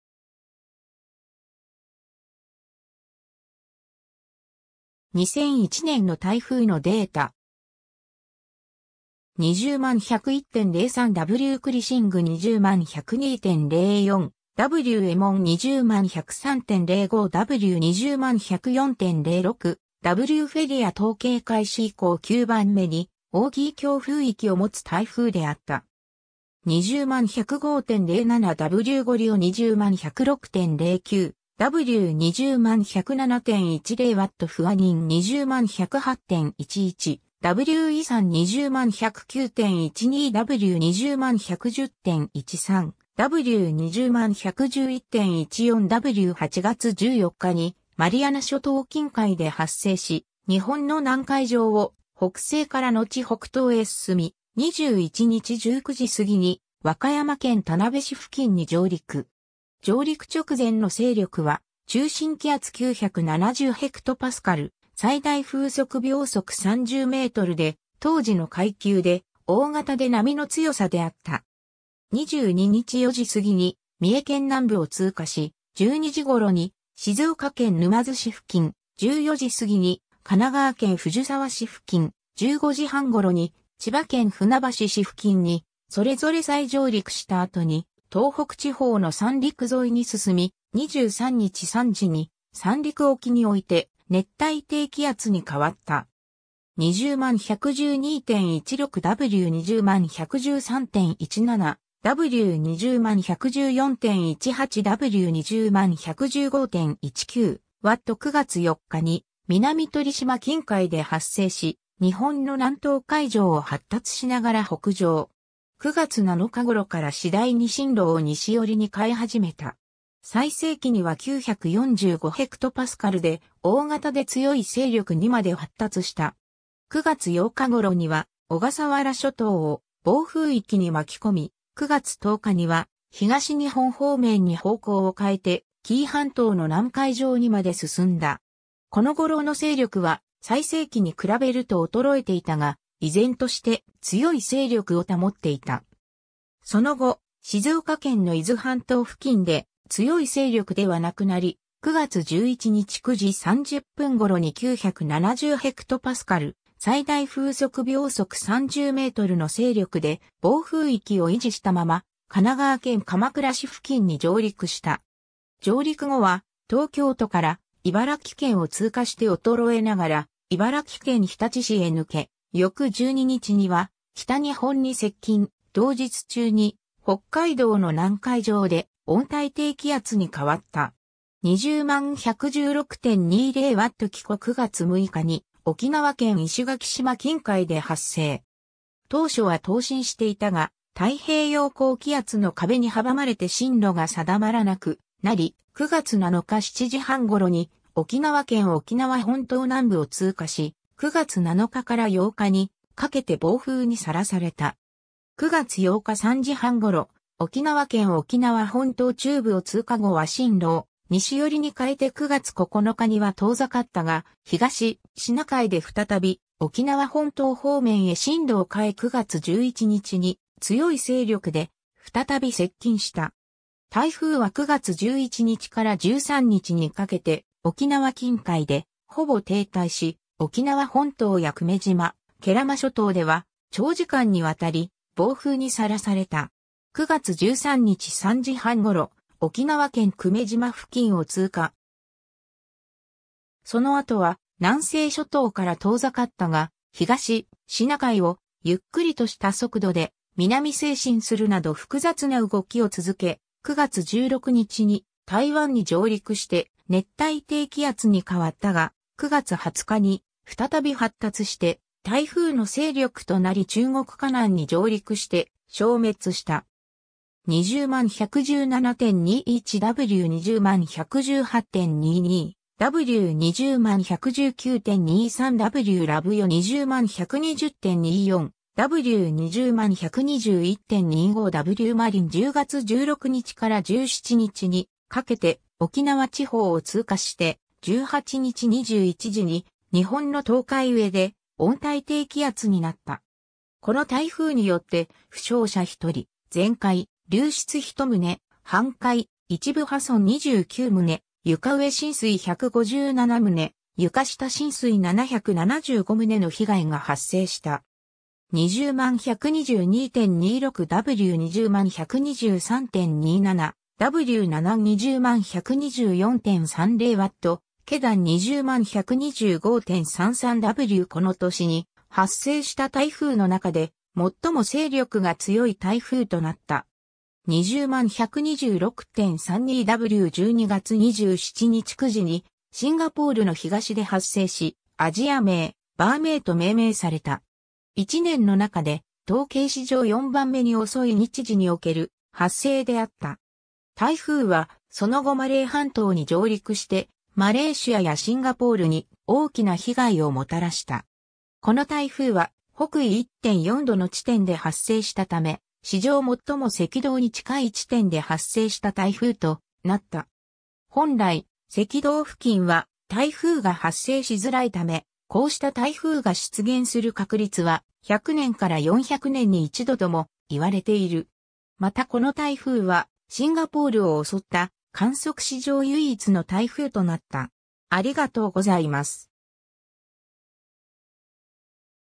2001年の台風
2001年の台風（2001ねんのたいふう、太平洋北西部で発生した熱帯低気圧）のデータ。
 200101・03W・クリシング 200102・04W・エモン 200103・05W 200104・06W・フェリア 統計開始以降9番目に大きい強風域を持つ台風であった。
 200105・07W・ゴリオ 200106・09W 200107・10W・フアニン 200108・11W・イサン 200109・12W 200110・13W 200111・14W 8月14日にマリアナ諸島近海で発生し、日本の南海上を北西からのち北東へ進み、21日19時過ぎに和歌山県田辺市付近に上陸。
上陸直前の勢力は中心気圧970hPa・最大風速30m/sで、当時の階級で「大型で並の強さ」であった。
22日4時過ぎに三重県南部（志摩半島付近）を通過し、12時頃に静岡県沼津市付近、14時過ぎに神奈川県藤沢市付近、15時半頃に千葉県船橋市付近にそれぞれ再上陸した後に東北地方の三陸沿いに進み、23日3時に三陸沖において熱帯低気圧に変わった。
 200112・16W 200113・17W 200114・18W 200115・19W 9月4日に南鳥島近海で発生し、日本の南東海上を発達しながら北上。
9月7日頃から次第に進路を西寄りに変え始めた。
最盛期には945hPaで「大型で強い勢力」にまで発達した。
9月8日頃には小笠原諸島を暴風域に巻き込み、9月10日には東日本方面に方向を変えて紀伊半島の南海上にまで進んだ。
この頃の勢力は最盛期に比べると衰えていたが、依然として「強い勢力」を保っていた。
その後、静岡県の伊豆半島付近で「強い勢力」では無くなり、9月11日9時30分頃に970hPa、最大風速30m/sの勢力で暴風域を維持したまま神奈川県鎌倉市付近に上陸した。
上陸後は東京都から茨城県を通過して衰えながら茨城県日立市へ抜け、翌12日には北日本に接近、同日中に北海道の南海上で温帯低気圧に変わった。
 200116・20W・キコ 9月6日に沖縄県石垣島近海で発生。
当初は東進していたが、太平洋高気圧の壁に阻まれて進路が定まらなくなり、9月7日7時半頃に沖縄県沖縄本島南部を通過し、9月7日から8日にかけて暴風にさらされた。
9月8日3時半頃、沖縄県沖縄本島中部を通過後は進路を西寄りに変えて9月9日には遠ざかったが、東シナ海で再び沖縄本島方面へ進路を変え9月11日に強い勢力で再び接近した。
台風は9月11日から13日にかけて沖縄近海でほぼ停滞し、沖縄本島や久米島・慶良間諸島では長時間にわたり暴風にさらされた。
9月13日3時半頃、沖縄県久米島付近を通過。
その後は南西諸島から遠ざかったが、東シナ海をゆっくりとした速度で南西進するなど複雑な動きを続け、9月16日に台湾に上陸して熱帯低気圧に変わったが、9月20日に再び発達して台風の勢力となり中国華南に上陸して消滅した。
 200117・21W 200118・22W 200119・23W・ラブヨ 200120・24W 200121・25W・マリン 10月16日から17日にかけて沖縄地方を通過して、18日21時に日本の東海上で温帯低気圧になった。
この台風によって、負傷者1人、全壊・流失1棟、半壊・一部破損29棟、床上浸水157棟、床下浸水775棟の被害が発生した。
 200122・26W 200123・27W・ナナン 200124・30W・ケダン 200125・33W この年に発生した台風の中で最も勢力が強い台風となった。
 200126・32W 12月27日9時（JST）にシンガポールの東で発生し、アジア名「ヴァーメイ（Vamei）」と命名された。
1年の中で、統計史上4番目に遅い日時における発生であった。
台風はその後マレー半島に上陸して、マレーシアやシンガポールに大きな被害をもたらした。
 この台風は北緯1.4度（赤道から150km）の地点で発生したため、史上最も赤道に近い地点で発生した台風となった。
本来、赤道付近は台風が発生しづらいため、こうした台風が出現する確率は、100年から400年に1度ともいわれている。
またこの台風は、シンガポールを襲った観測史上唯一の台風となった。